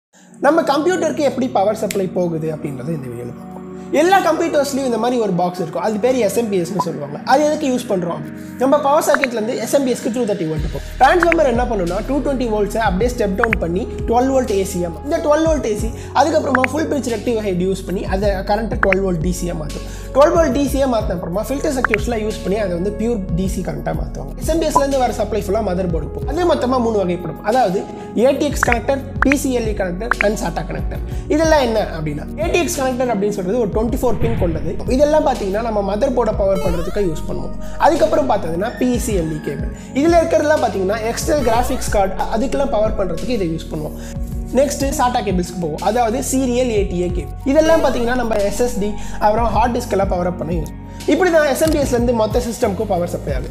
Power System unit in Computer
In this video, I have explained about how does the Power transfer in Computer from our Power sorket to SMPS and how it supplied to entier Computer. Here I have also talked about ATX connectors, PCIe connectors, SATA connectors and what they supplies power